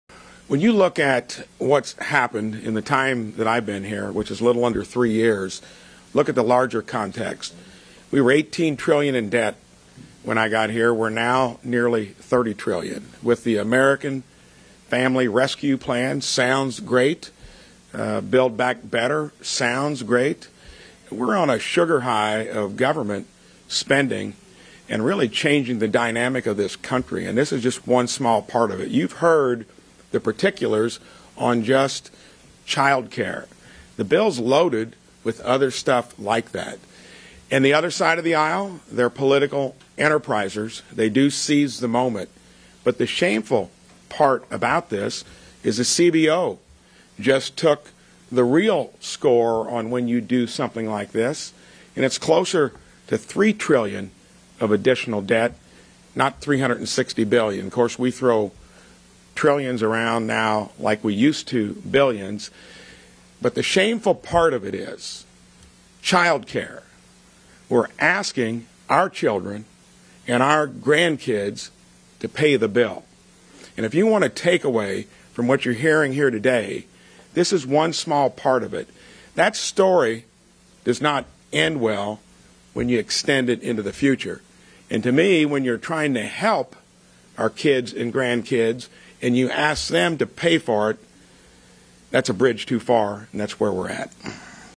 'Sugar High Of Government Spending': GOP Senator Hammers Dems Over Build Back Better
At a press briefing on Wednesday, Sen. Mike Braun (R-IN) spoke about Build Back Better.